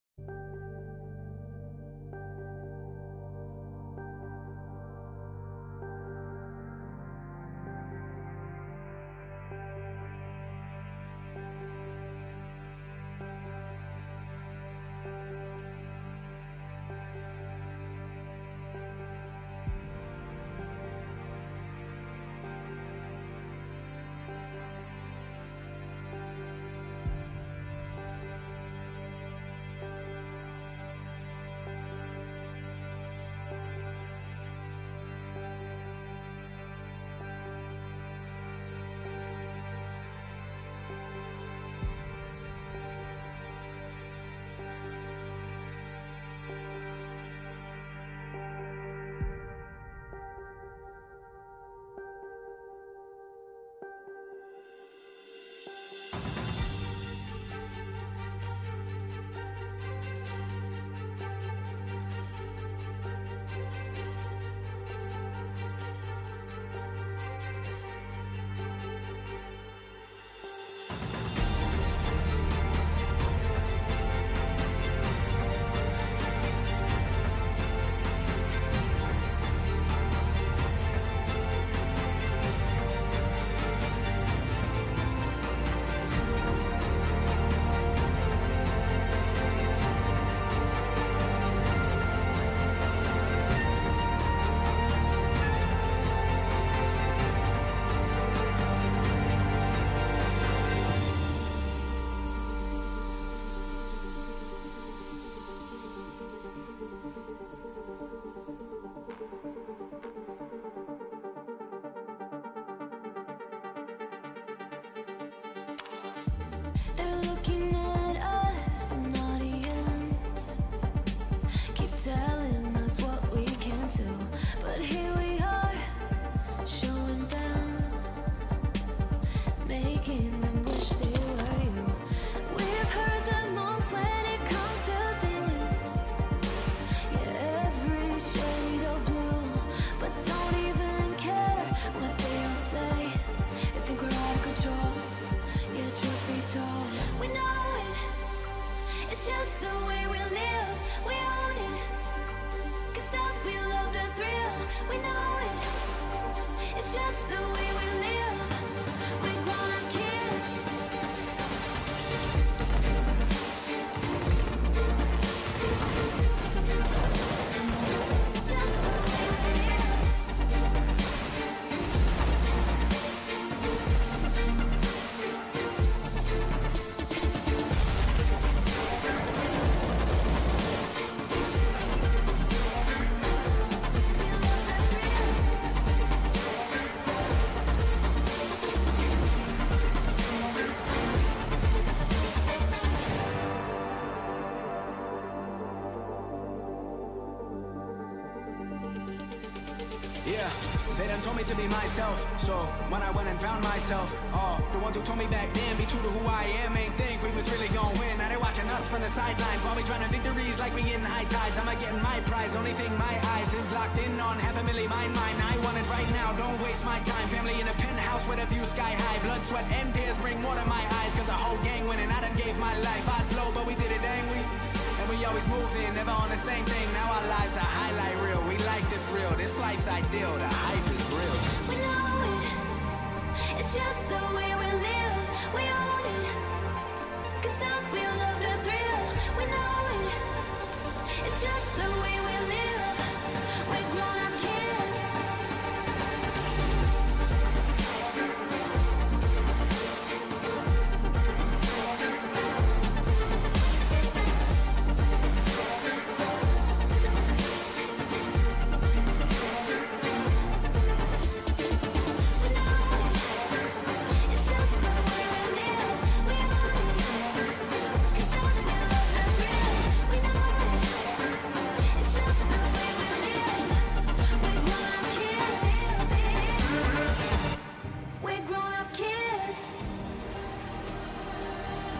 IEM Katowice 2020 - Semi-Finals Opening Ceremony by Nause feat. AK
Swedish house DJ Nause made a huge track for this year's IEM Katowice Semi-finals. Let us know, if you like it!

Support the music Artist!